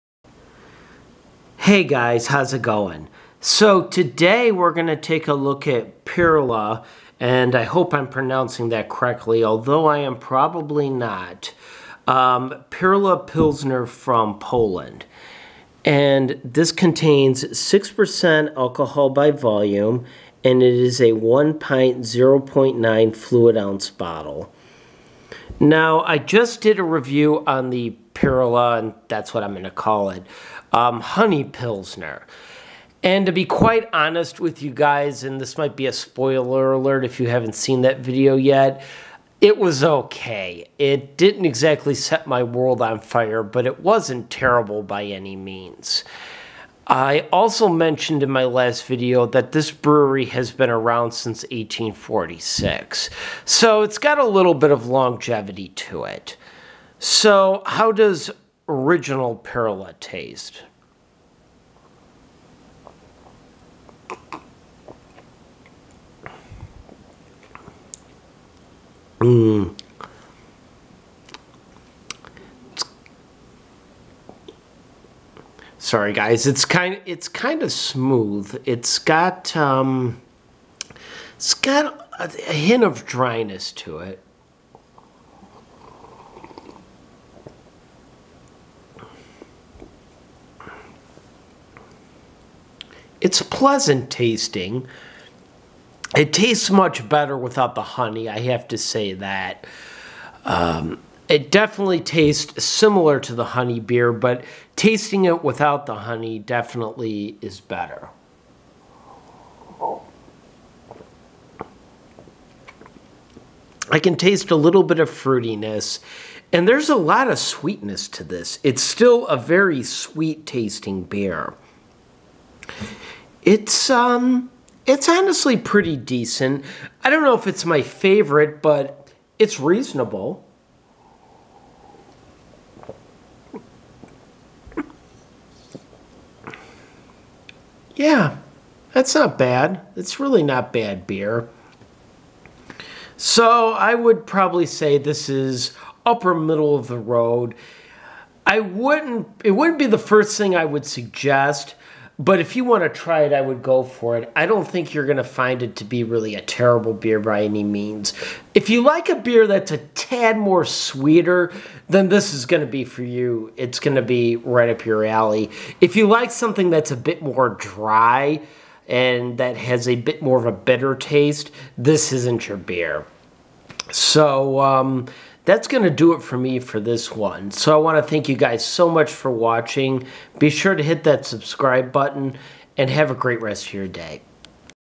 Perla Chmielowa Pils Beer Review
I like this much better then the honey flavor.